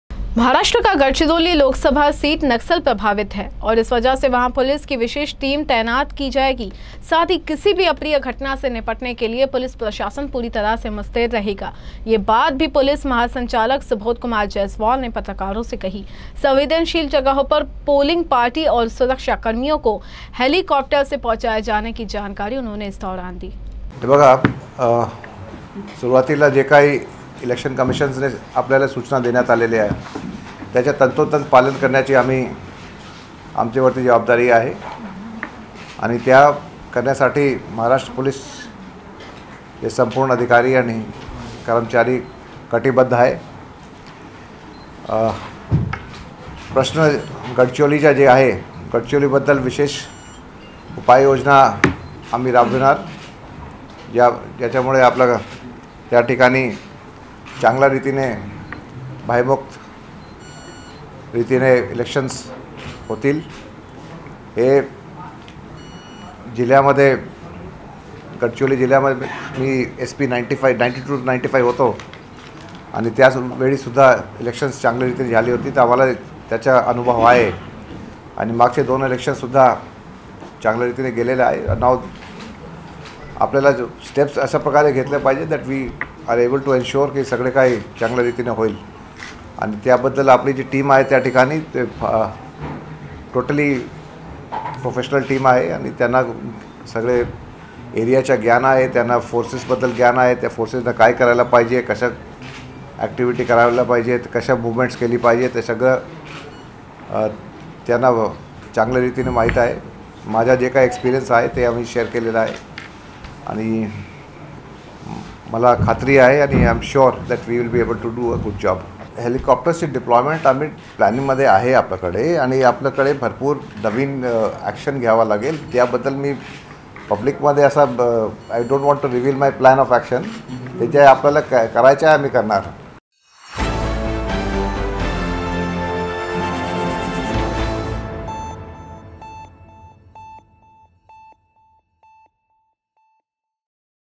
चुनाव के दौरान गडचिरोली में रहेंगी पोलिस के विशेष टीम की तैनाती
यूसीएन न्यूज़